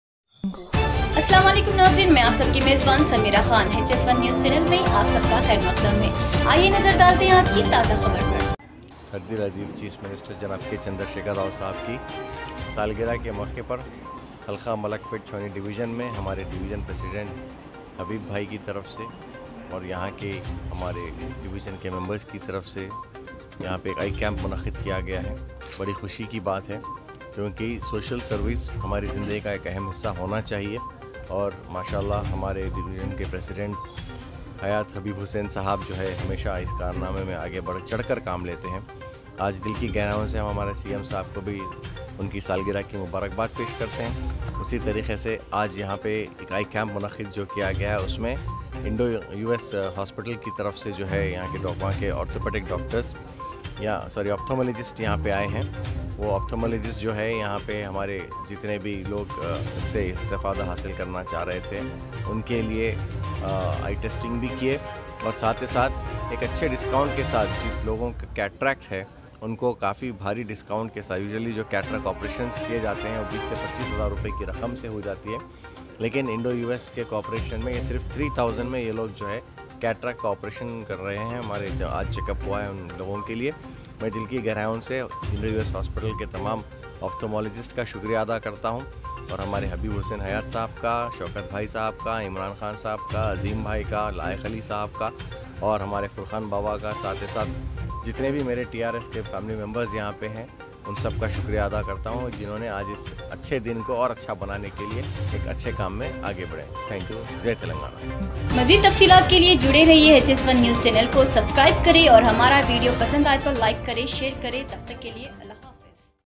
CM KCR KI PAIDAYESH KE MAUQE PAR DAWN HIGH SCHOOL MALAKPET ME FREE EYE CAMP MUNAQID KIYA GAYA..
On the occasion of birthday of Telangana chief minister today . Free Eye camp conducted by trs party chawani division. Organised by division president hayath hussain habeeb . . Contested coperaotor mohd shokat .chief guest trs party malekpet incharge azam ali kuram . And others leaders were present. *@HS1 NEWS HYDERABAD